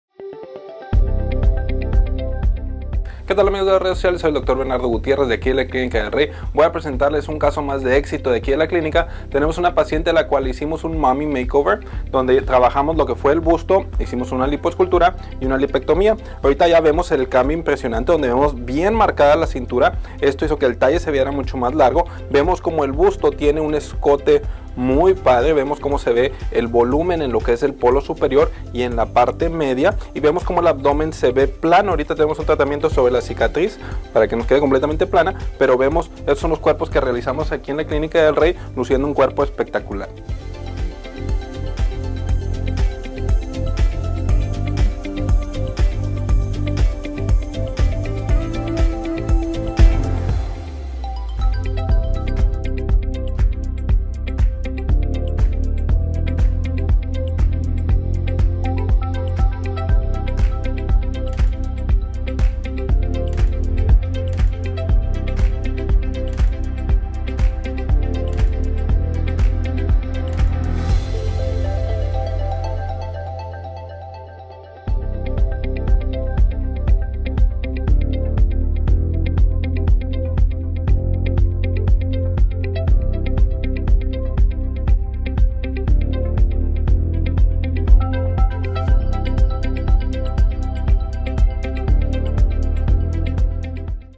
MOMMY MAKEOVER | Dr. Franco Reyes Jácome | Clínica del Rey
El mommy makeover es un conjunto de intervenciones quirúrgicas - 3 para ser exactos - que permite a las mujeres que acaban de convertirse en madres recuperar su figura previa al embarazo, o incluso mejorar su silueta. la ventaja de este procedimiento es que permite una recuperación y unos resultados más rápidos, así como un ahorro de dinero, al combinar varias intervenciones

usuario: yoconjacome

usuario: dr. franco reyes jacome plastic surgeon

UBICACIÓN: Calle 5ta esq. Ave. Álvaro Obregón #1809 H. Matamoros Tamaulipas. México